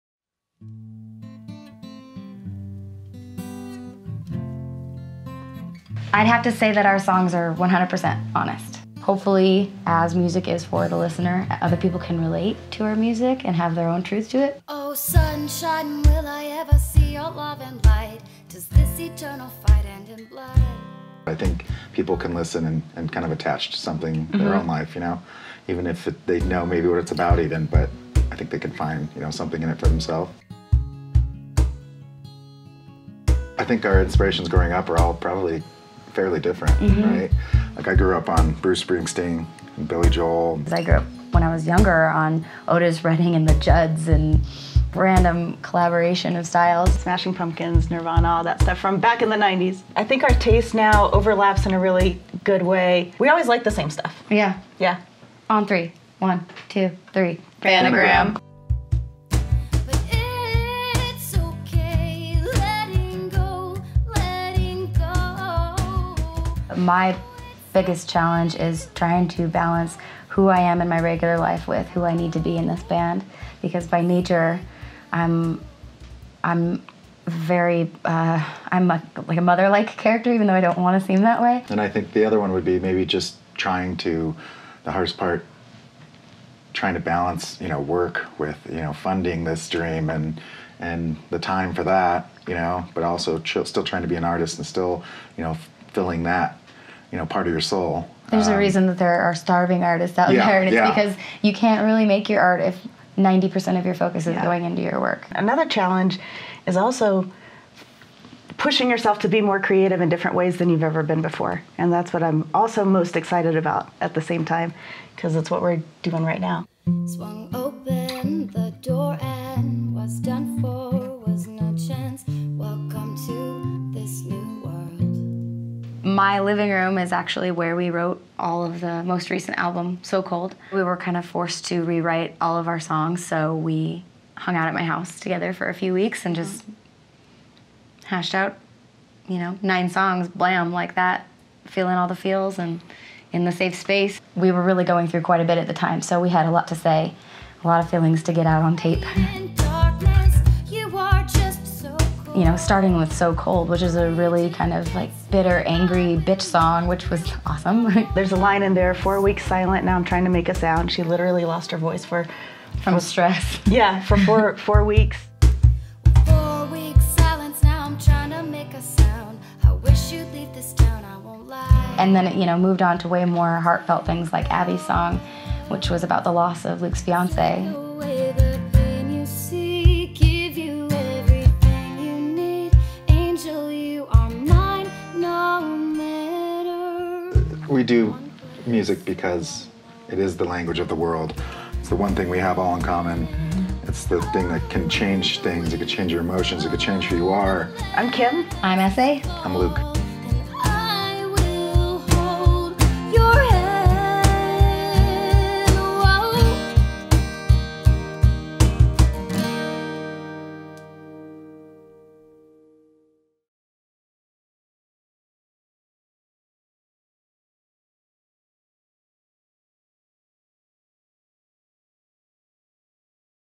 LIPTRUCE
A trio of musicians from vastly different musical backgrounds come together to create songs of intimate honesty.